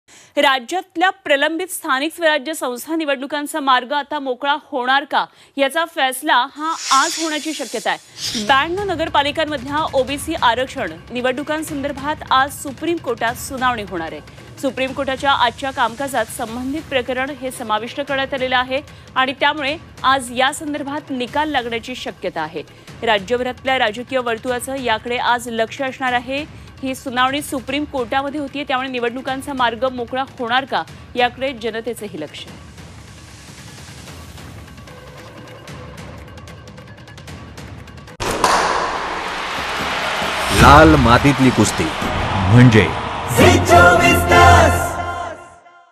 SC Hearing on OBC Reservation | ओबीसी आरक्षणावर येणार फैसला? निवडणुकांचा होणार मार्ग मोकळा?
Supreme Court Hearing On OBC Reservation And Local Celf Government Election
राज्यातल्या प्रलंबित स्थानिक स्वराज्य संस्था निवडणुकांचा मार्ग मोकळा होणार का याचा फैसला आज होण्याची शक्यता आहे. 92 नगरपालिकांमधील ओबीसी आरक्षण, निवडणुकांसंदर्भात आज सुप्रीम कोर्टात सुनावणी होणार आहे. सुप्रीम कोर्टाच्या आजच्या कामकाजात संबंधित प्रकरण समाविष्ट करण्यात आलंय.  आहे. त्यामुळे आज या संदर्भात निकाल लागण्याची शक्यता आहे.
A decision is likely to be taken today as to whether the pending local body elections in the state will be cleared. The hearing will be held today in the Supreme Court regarding OBC reservation, elections in 92 municipalities. The relevant case has been included in today's proceedings of the Supreme Court. is Therefore, the result is likely to be announced today in this regard.